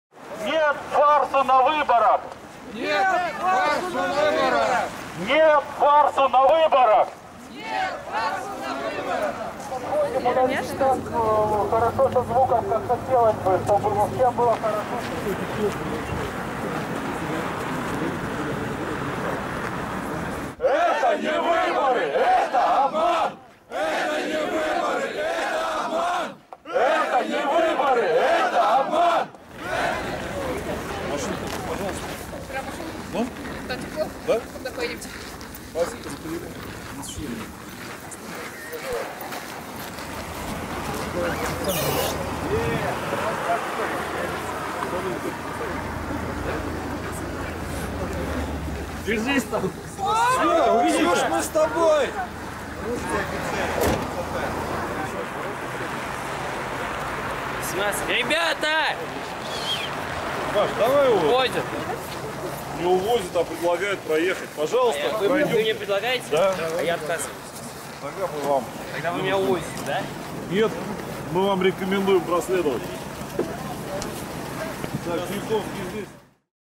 Tomsk: Hapšenje pristalica Alekseja Navaljnog
Ruska policija privela je nekoliko učesnika skupa koji su podržali antikorupcijskog aktivistu i opozicionara Alekseja Navaljnog u gradu Tomsku. Demonstranti u tom gradu marširali su ulicama uzvikujući da se protive izbornoj farsi - misleći na martovske predsdedničke izbore. Video: Ruski servis RSE